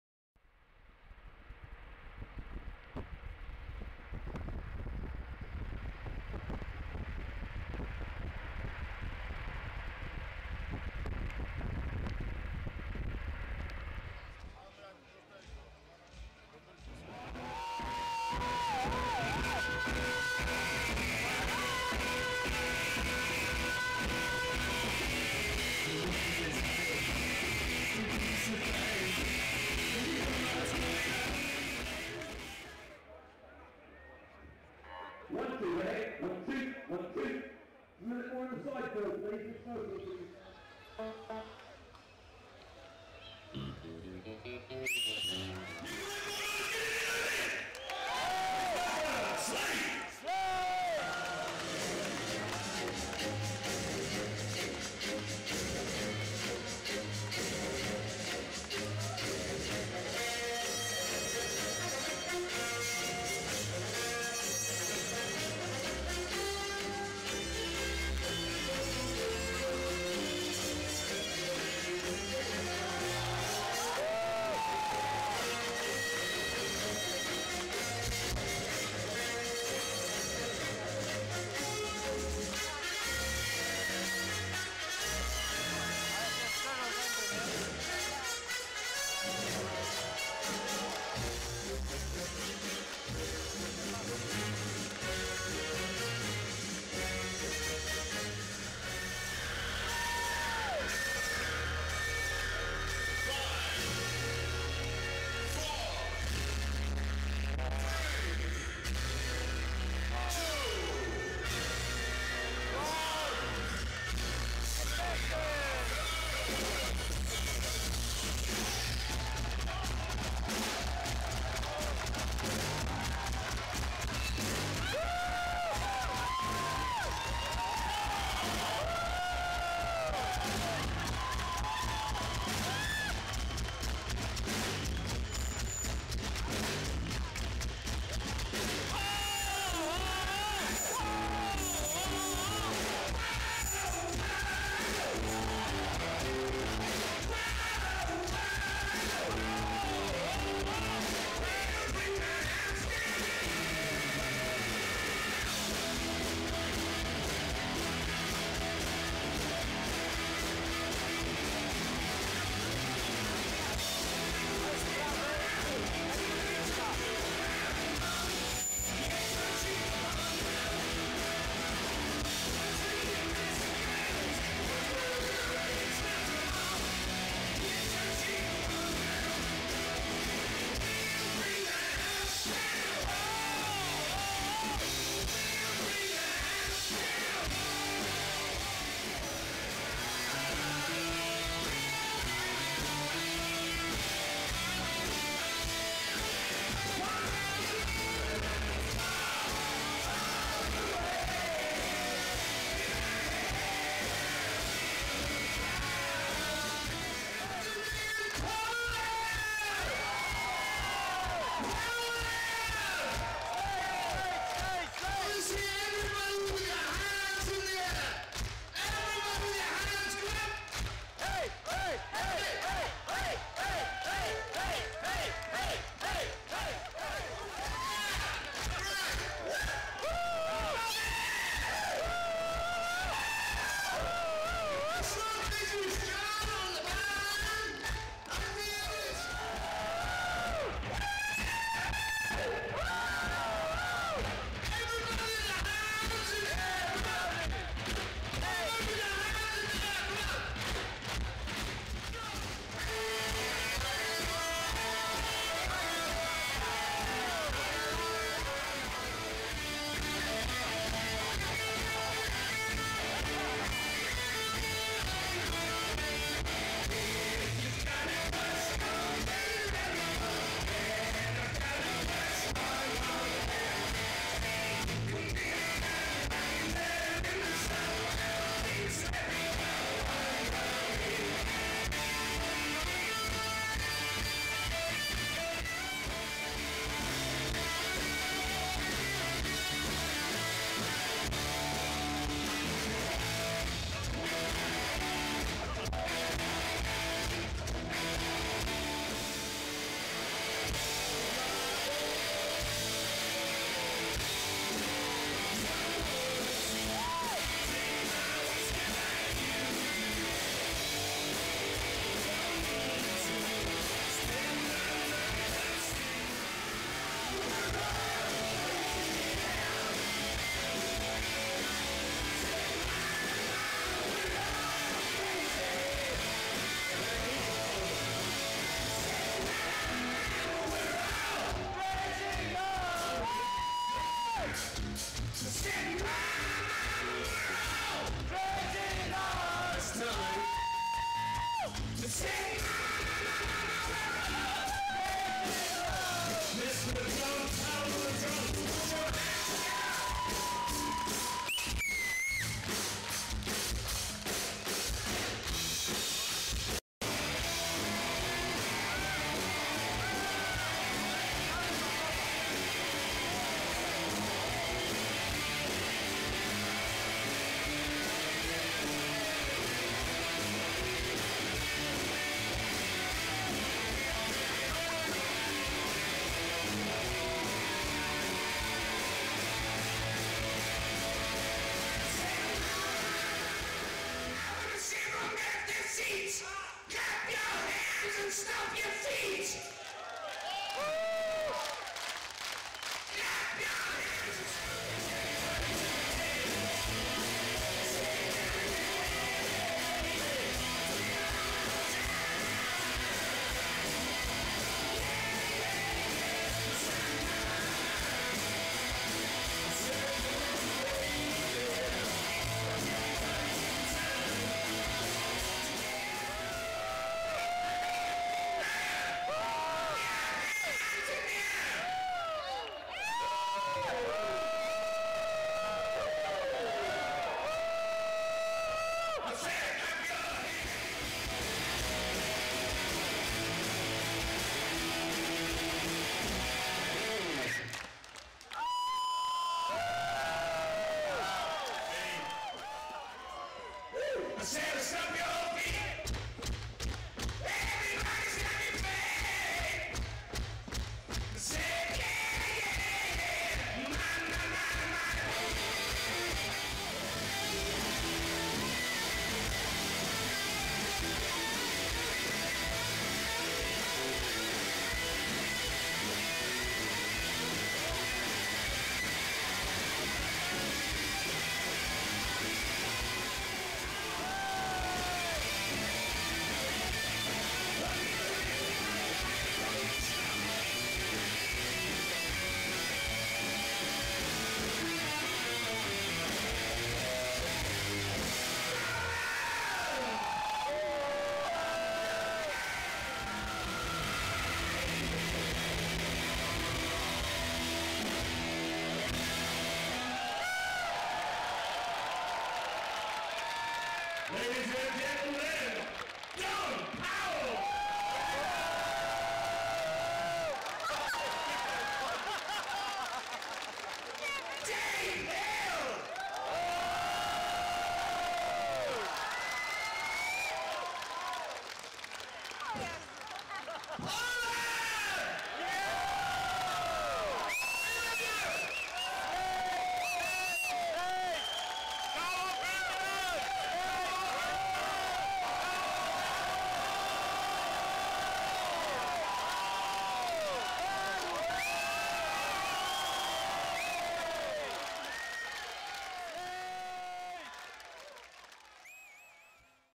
SLADE 2011 In Poland Lubin part 1
Koncert na Jubileuszu KGHM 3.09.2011r. cz. 1